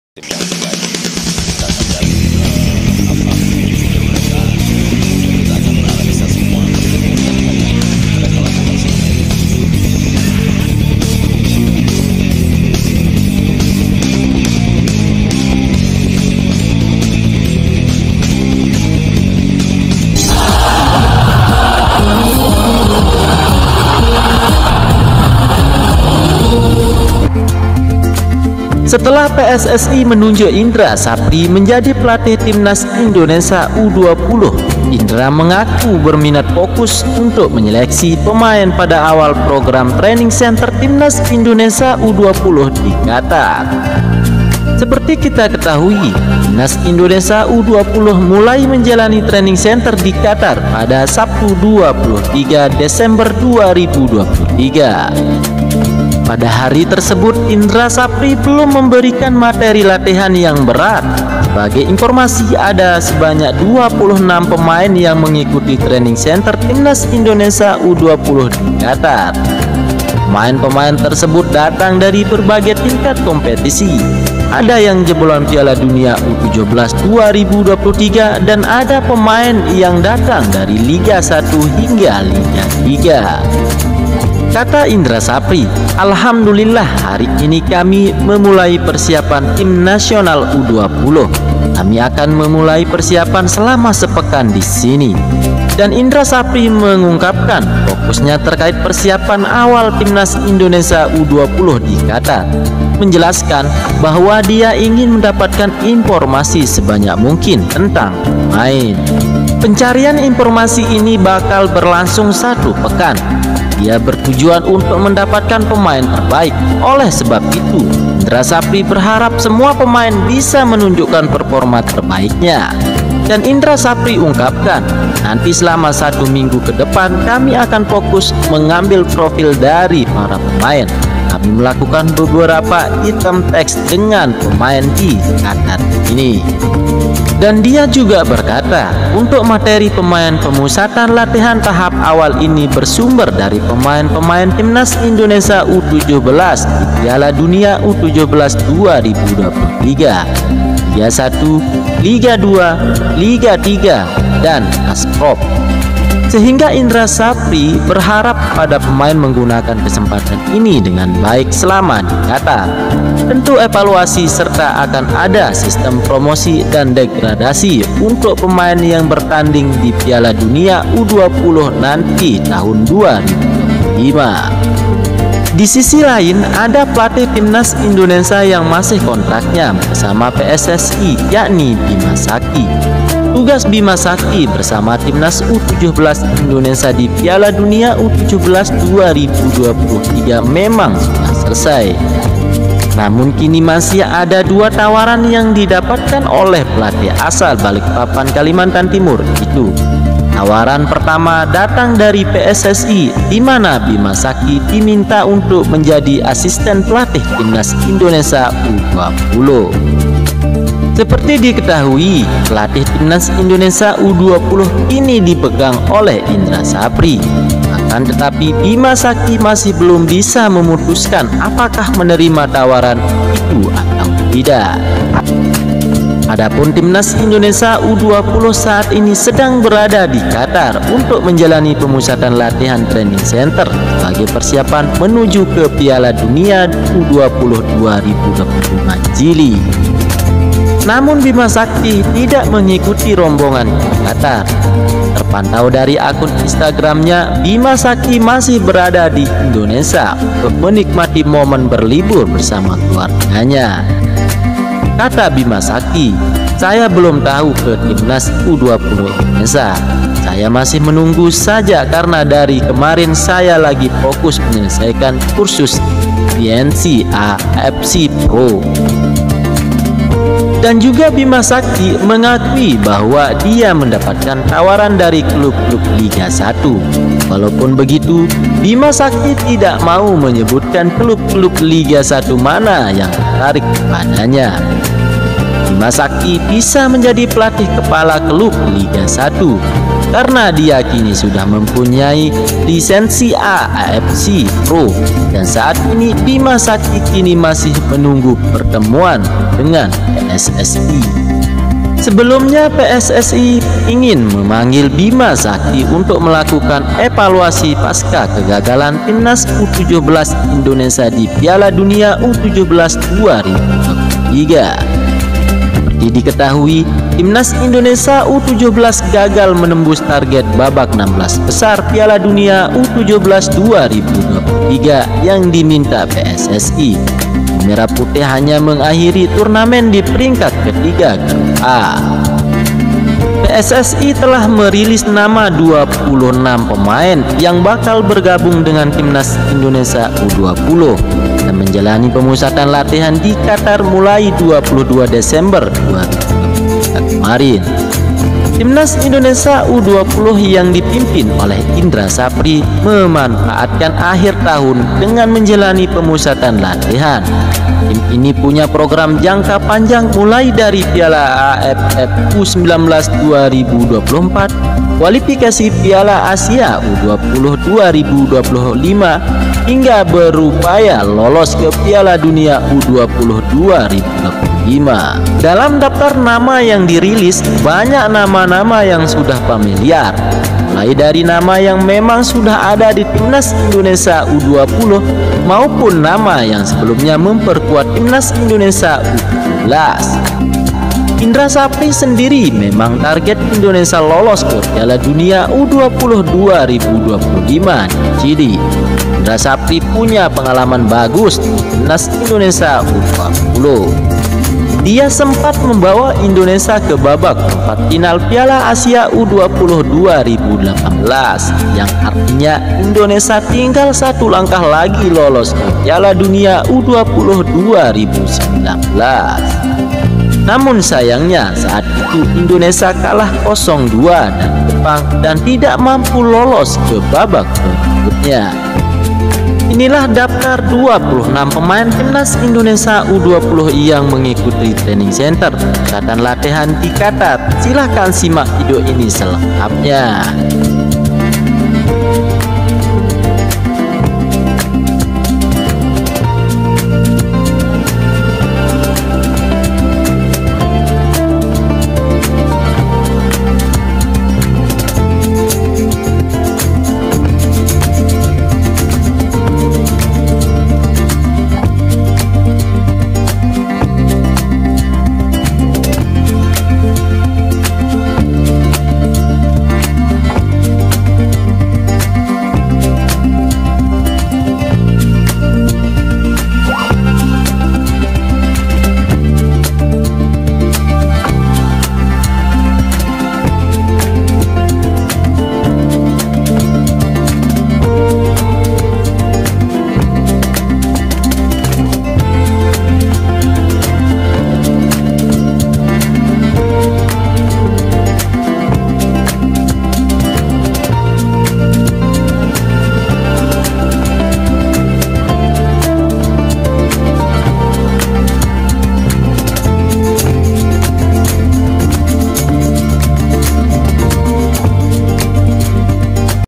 PSSI Tarwarkan Bima Sakti Menjadi Asisten Pelatih Indra Sjafri di Timnas Indonesia U-20 Untuk PILDUN
Highlight Indonesia vs Libya tadi malam
hasil Indonesia vs Libya tadi malam
uji coba Indonesia vs Libya
Libya vs Indonesia
ada dua tawaran yang didapatkan oleh pelatih asal Balikpapan, Kalimantan Timur, itu.Tawaran pertama datang dari PSSI, dimana Bima Sakti diminta untuk menjadi asisten pelatih timnas Indonesia U-20

Seperti diketahui, pelatih timnas Indonesia
U-20 kini dipegang oleh Indra Sjafri. , akan
tetapi Bima Sakti masih belum bisa
memutuskan apakah menerima tawaran itu
atau tidak.

Adapun timnas Indonesia U-20  saat ini
sedang berada di Qatar untuk menjalani
pemusatan latihan (TC) sebagai persiapan
Menuju ke Piala Dunia U-20 2025 Chile.

Namun Bima Sakti tidak mengikuti rombongan ke Qatar.Terpantau dari akun instagramnya, Bima Sakti masih berada di Indonesia untuk menikmati momen berlibur bersama keluarganya.

kata Bima Sakti"Saya belum tahu (ke timnas
U-20 Indonesia)," "Saya masih menunggu
saja karena dari kemarin saya lagi fokus
menyelesaikan kursus lisensi A AFC Pro,"

Dan juga Bima Sakti mengakui bahwa dia
mendapatkan tawaran dari klub-klub Liga 1.
Walaupun begitu, Bima Sakti tidak mau
menyebutkan klub Liga 1 mana yang tertarik
kepadanya.

Bima Sakti bisa menjadi pelatih kepala klub
Liga 1.karena, dis kini sudah mempunyai
lisensi A AFC Pro.Dan saat ini Bima Sakti kini
masih menunggu pertemuan dengan PSSI.

Sebelumnya, PSSI ingin memanggil Bima Sakti untuk melakukan evaluasi pasca kegagalan timnas U-17 Indonesia di Piala Dunia U-17
2023

Seperti diketahui, timnas Indonesia U-17 gagal
menembus target babak 16 besar Piala Dunia
U-17 2023 yang diminta PSSI.Tim Merah Putih
hanya mengakhiri turnamen di peringkat
ketiga Grup A

Timnas Indonesia U-20 yang dipimpin oleh
Indra Sjafri memanfaatkan akhir tahun
dengan menjalani pemusatan latihan. Tim ini
punya program jangka panjang mulai dari
Piala AFF U-19 2024, Kualifikasi Piala Asia
U-20 2025 hingga berupaya lolos ke Piala
Dunia U-20 2025

Daftar 26 pemain Tim U-20 Indonesia di
pemusatan latihan Qatar

1. Ji Da Bin, ASIOP
2. Kadek Arel, Bali United
3. Azriel Aimar, Barito Putera
4. Alexandro Felix, Barito Putera
5. Muhammad Iqbal G, Barito Putera
6. Muhammad Iqbal S, Bhayangkara FC
7. Ferre Murari, Bhayangkara FC
8. Ramadhan Yusuf, Bhayangkara FC
9. Andrika Fathir, Borneo FC
10. Rizdjar Nurviat, Borneo FC
11. Hugo Samir, Borneo FC
12. Muhammad Kafiatur, Dewa United
13. Muhammad Riski A, Madura United
14. Muhammad Rafly I, Malut United
15. Dony Tri, Persija
16. Muhamad Ibrah, Persija
17. Arlyansyah Abdulmanan, Persija
18. Nathan Fariel, Persija
19. Muhammad Nabil, Persija
20. Muhammad Syamsul, Persikabo
21. Arkhan Kaka, Persis
22. Rifky Tofani, Malut United
23. Tonci Shouter, PPLP Papua
24. Sulthan Zaky, PSM
25. Muhammad Mufli, PSM
26. Ikram Algiffari, PPLP Sumbar